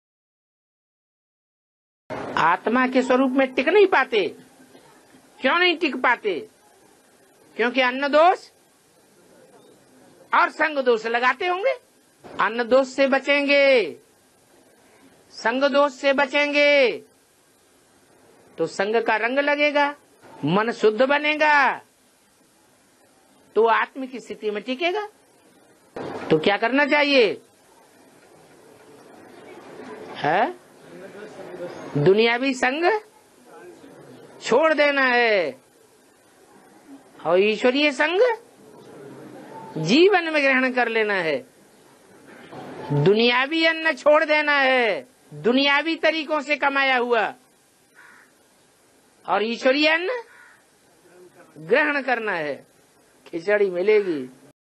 593 What hampers the soul conscious stage
593 What hampers the soul conscious stage

No Brahmakumar-kumari has the power to make an advance Brahmin into a basic Brahmin. (Student: No Baba, they have that power.) Where? (Student: the Bks.) Did they become advance? Did they recognise the advance Father? They didn’t recognise Him at all and those who go becoming firm in advance [knowledge], can the Bks incorporate them within them? They don’t have the power to incorporate them at all. They can’t digest (fig. adjust with) them. They will tear the stomach and come out. These are the children of the Father, [who are like] scorpions and spiders.